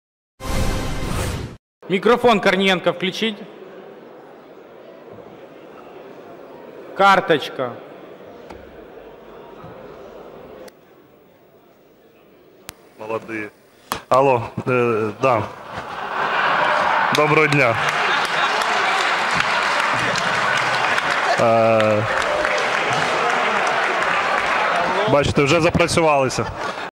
"Алло! Да!" - Корниенко перепутал в Раде телефон с микрофоном
Лидер партии Слуга народа Корниенко перепутал в Раде телефон с микрофоном